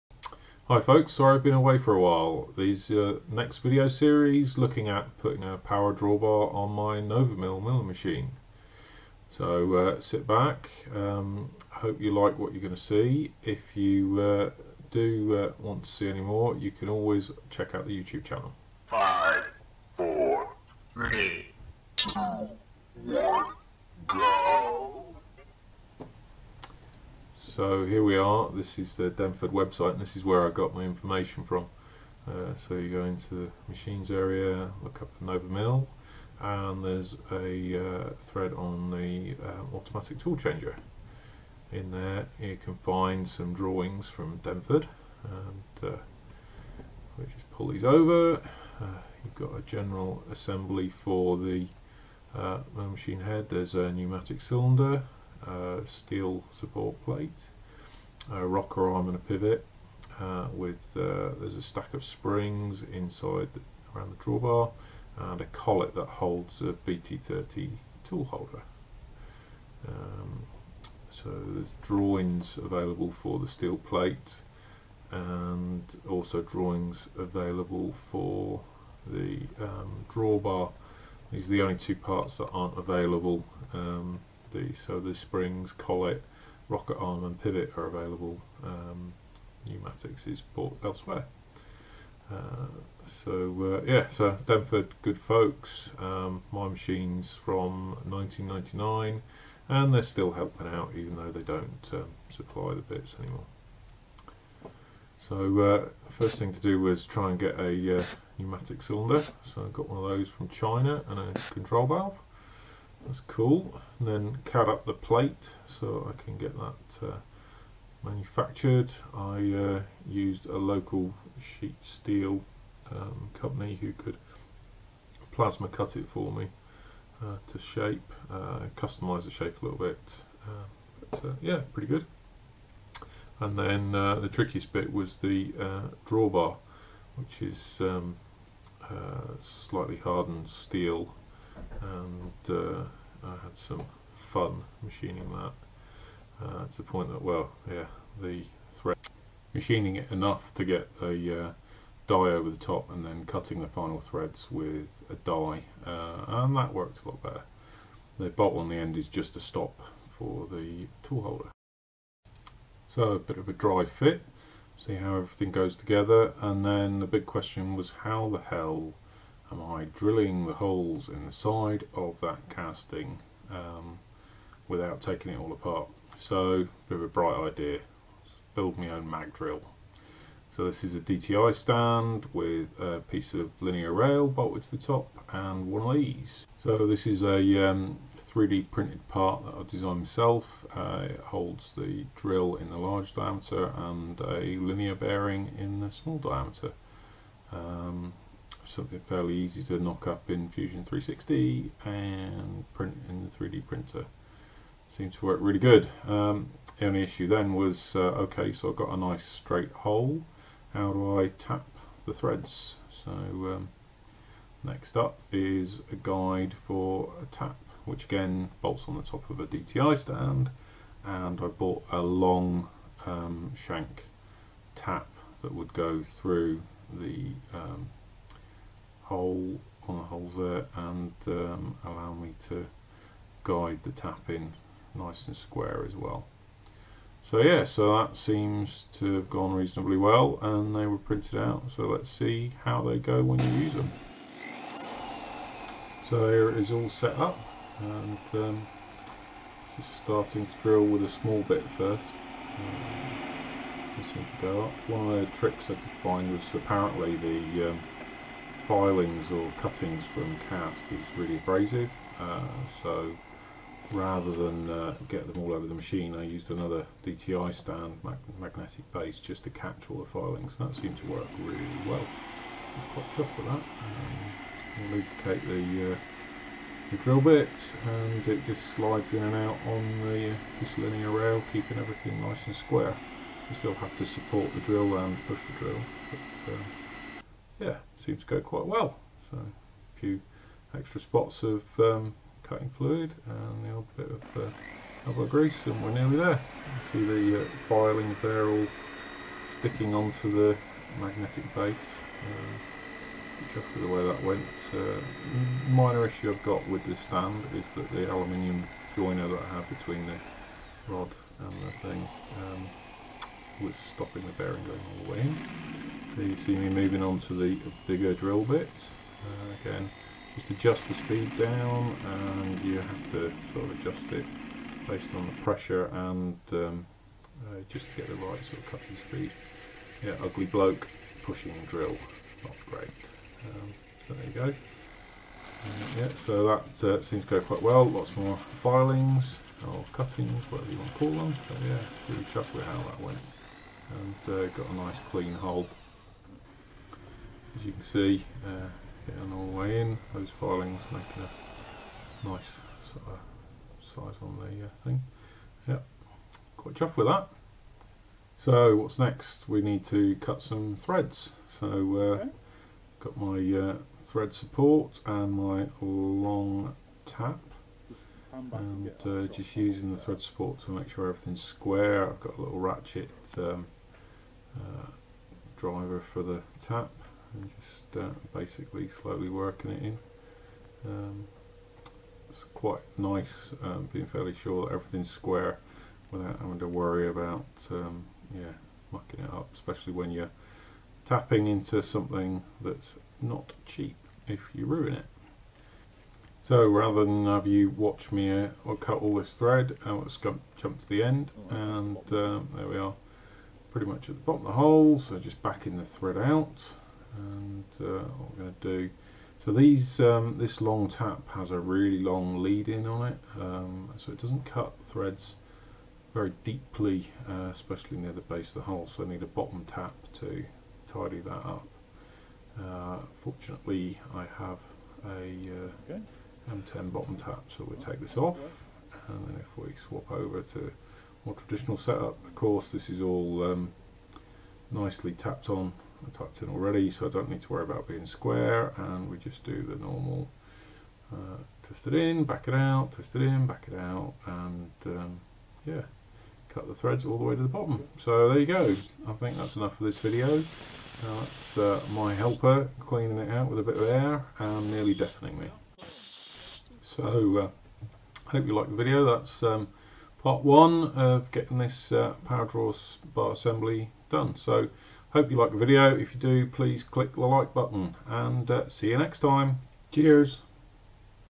DIY Denford Novamill CNC Milling Machine Power Draw-bar Upgrade #1
This video is the first in a series about retrofitting a power draw-bar to a Denford Novamill Milling Machine.